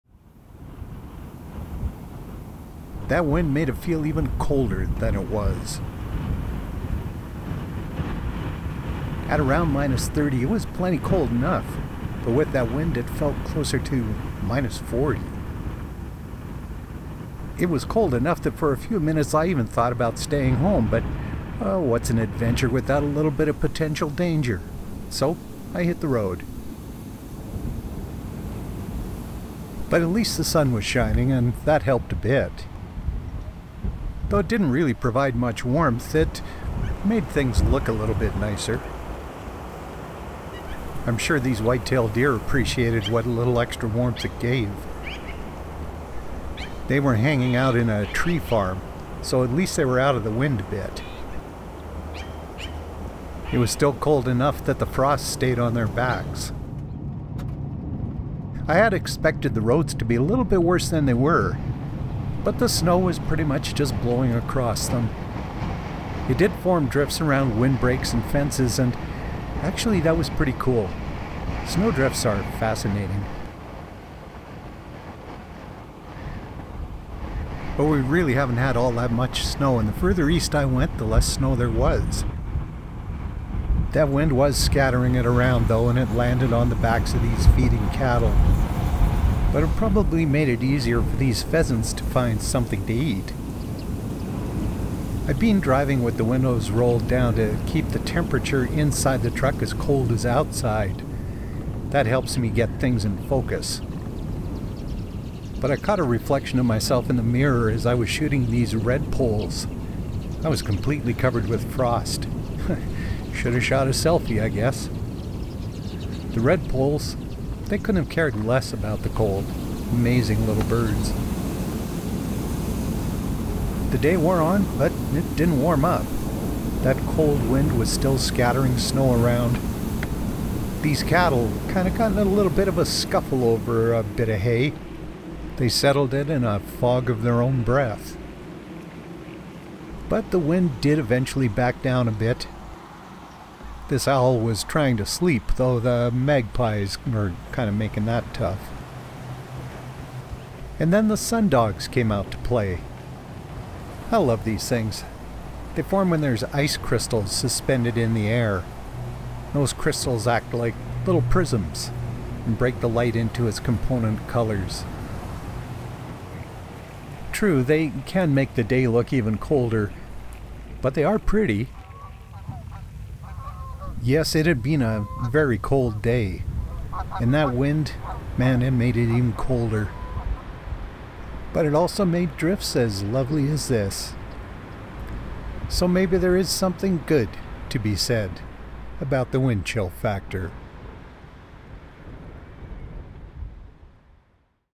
Wind Chill
It was already a cold day but that wind made it feel even colder. Still, it wasn't all bad.